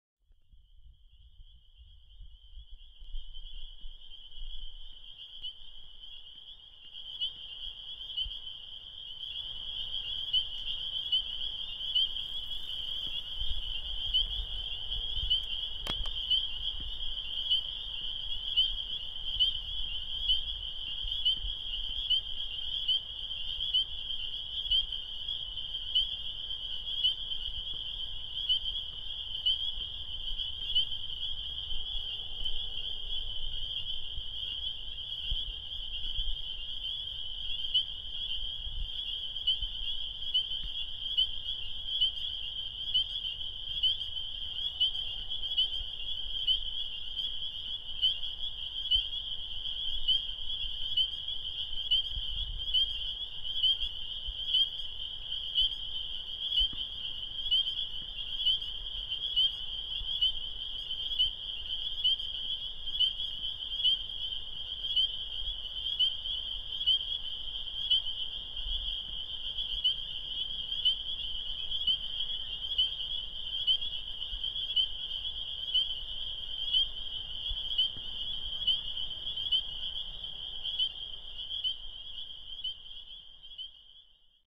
PEEPERS & CROCUSES 1080p
Spring peepers, Pseudacris, calling in wetlands in Montgomery County, Maryland accompanied by images of Tommies, Crocus tommasinianus. By Jim McKenney.